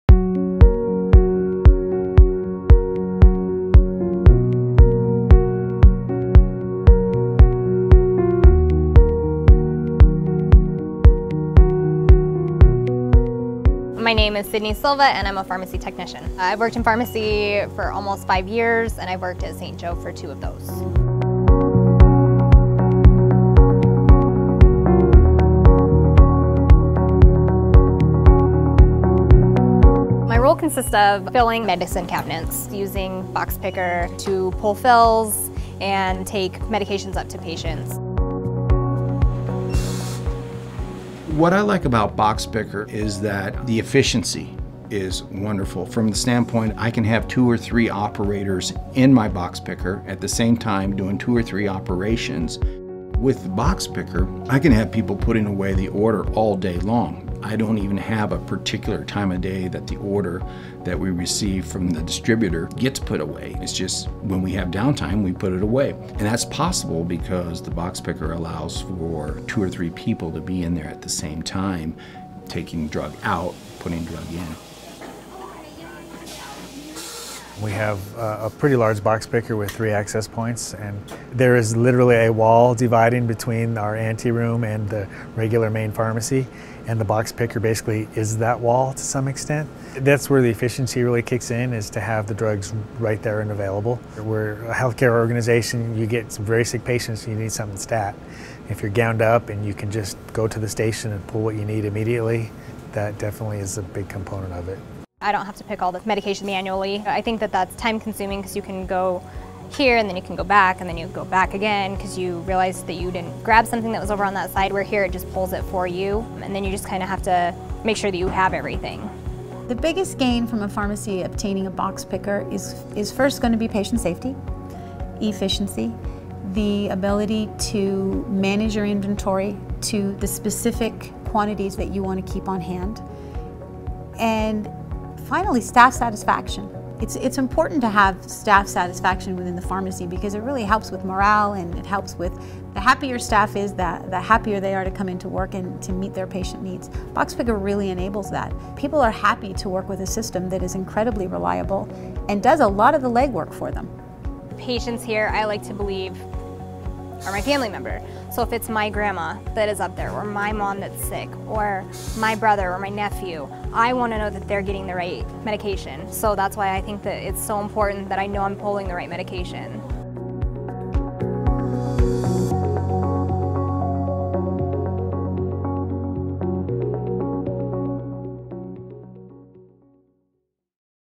BoxPicker - Improve Workflow with Fully Automated Storage
Discover how the St. Joseph Hospital pharmacy improved and streamlined its workflow and staff efficiency by fully automating their drug storage with BoxPicker.